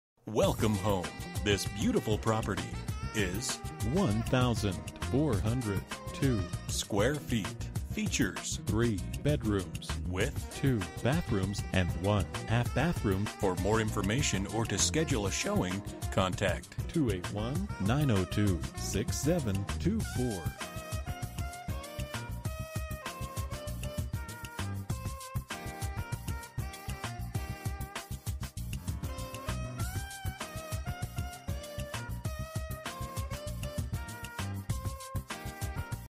Residential for sale - 9545 Pagewood Lane, Houston, TX 77063
Needs some TLC please call for you showing!!

Bedrooms: 3
Bathrooms: 2 full and 1 half
Square Feet: 1402
Price: $99,000

04/22/2019 07:38:52 pm

Last modified: